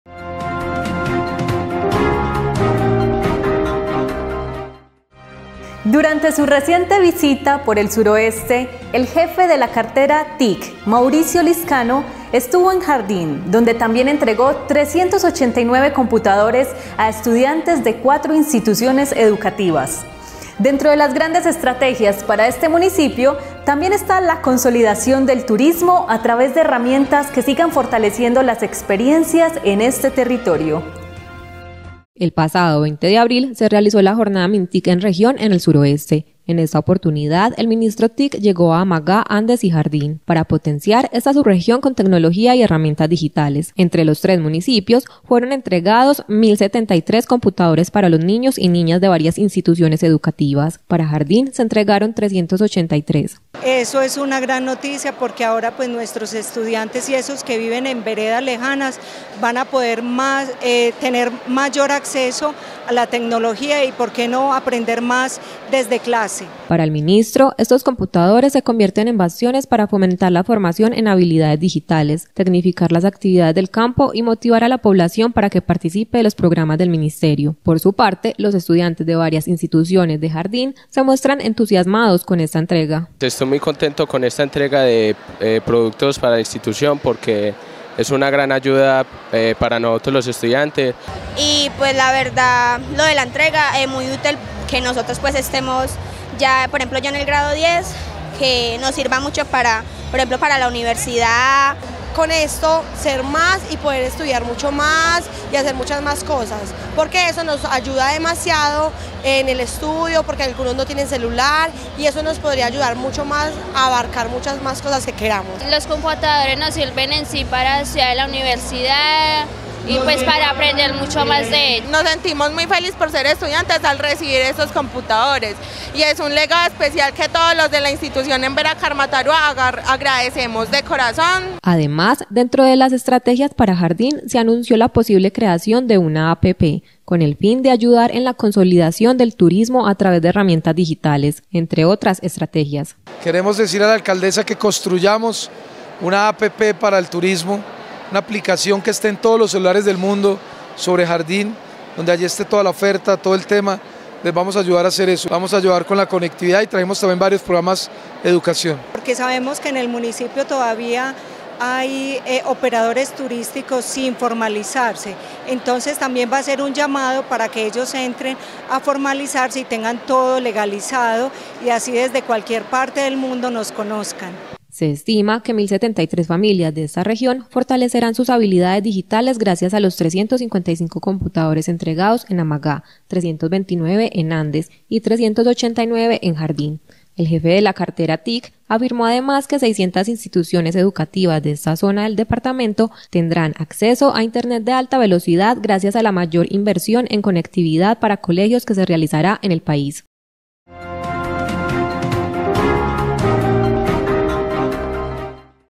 MINISTRO TIC ENTREGA COMPUTADORES EN JARDÍN
Durante su reciente visita por el suroeste, el jefe de la cartera TIC, Mauricio Lizcano estuvo en Jardín, donde también entregó 389 computadores a estudiantes de cuatro instituciones educativas.